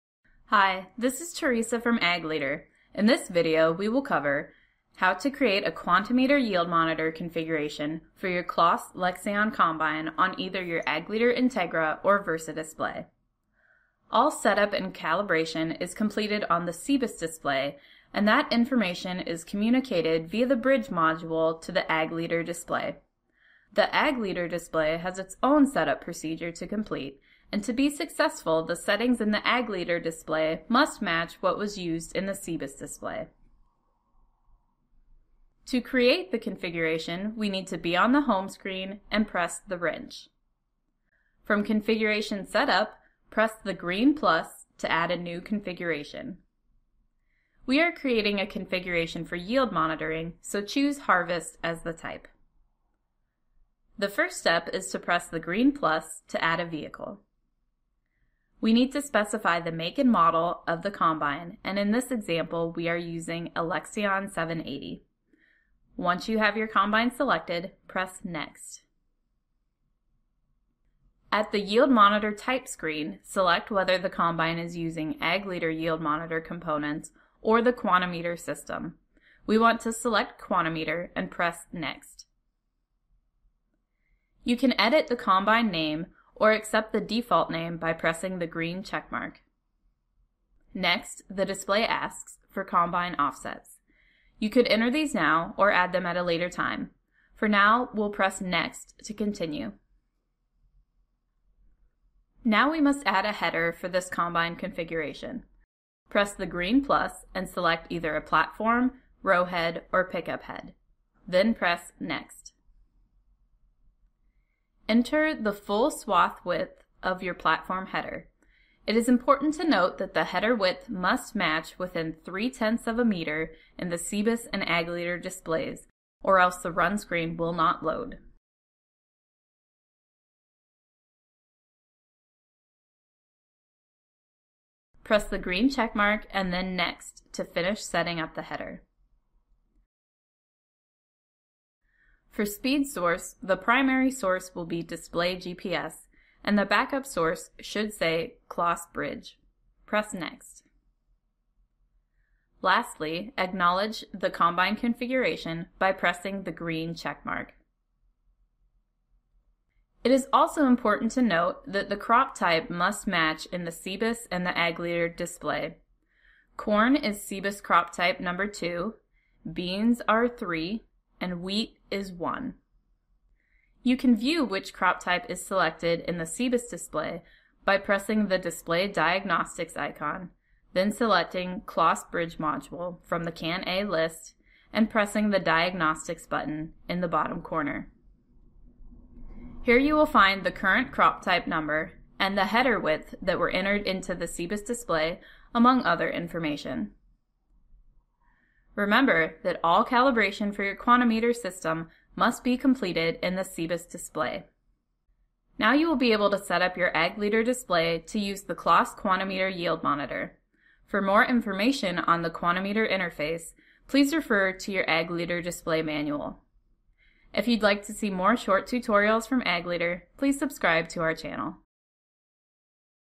Use the CLAAS Quantimeter yield monitor with your Ag Leader display
Learn how to create a Quantimeter yield monitor configuration for your CLAAS LEXION combine on either your Ag Leader Integra or Versa display.